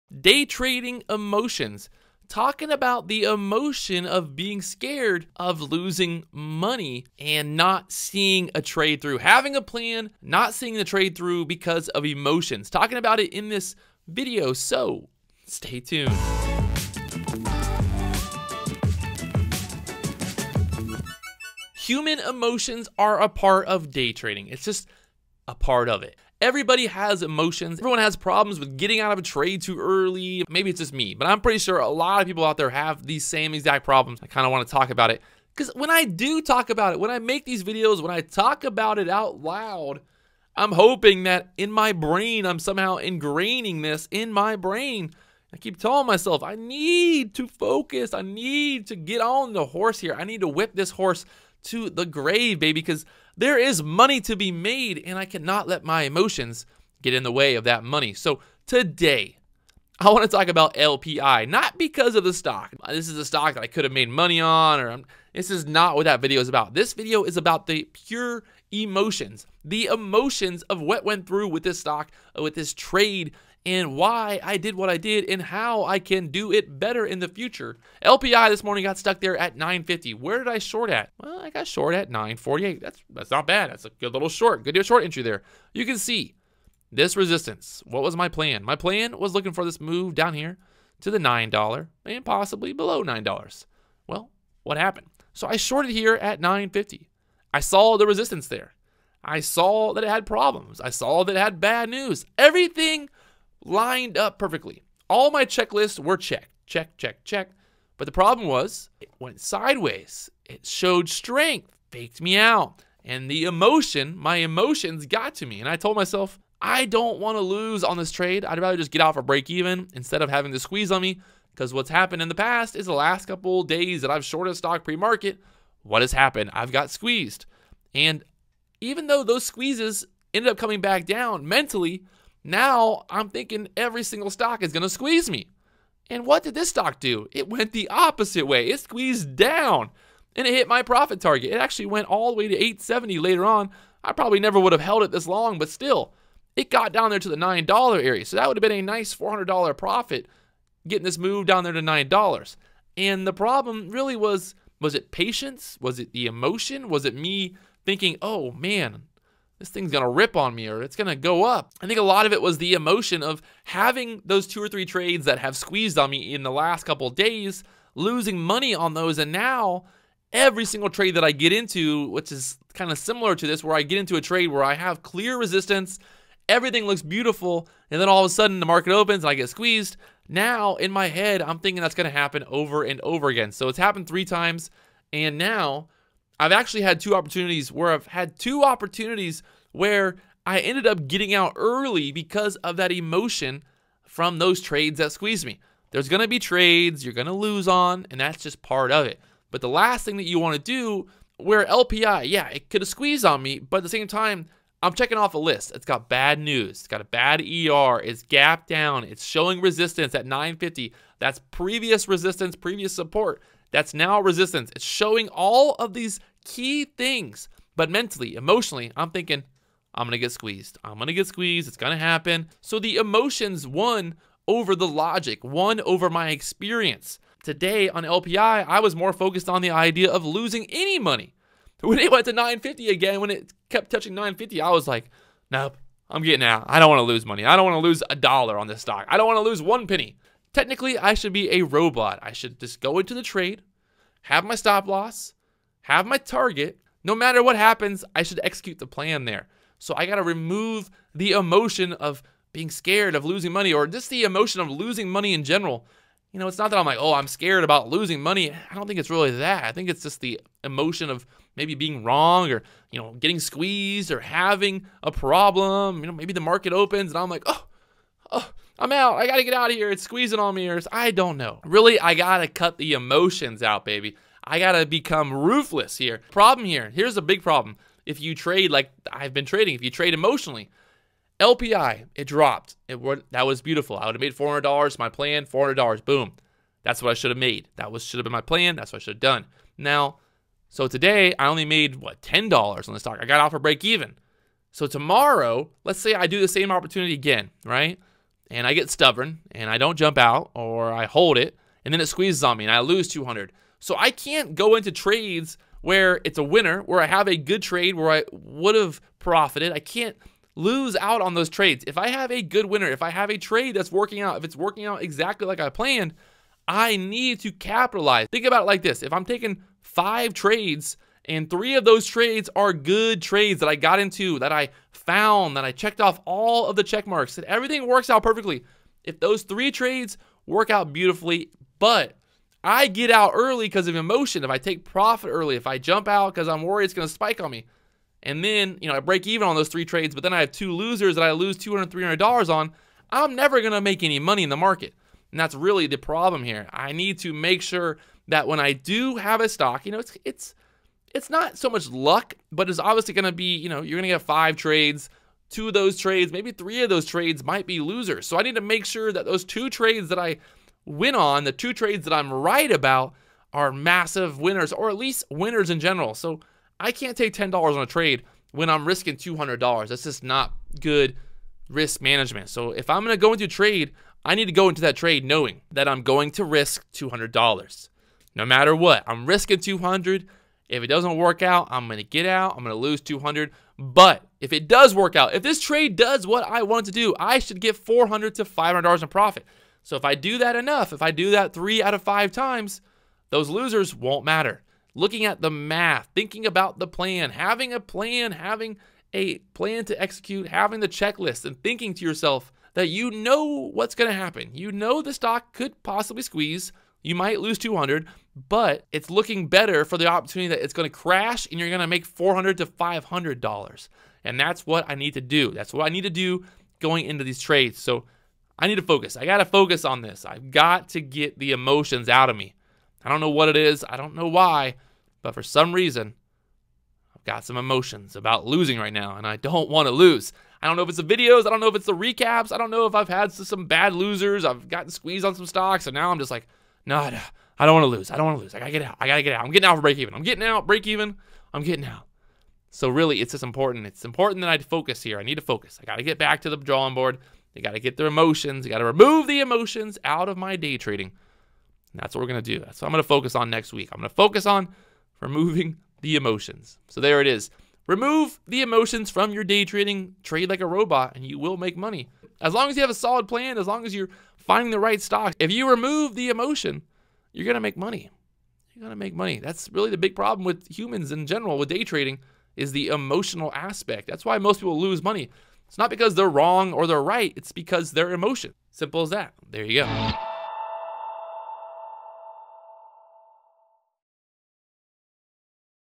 DAY TRADING! FEAR OF LOSING MONEY!
THE FEAR OF LOSING MONEY Day Trading! Lets talk about that fear and conquer it together. Right here right now!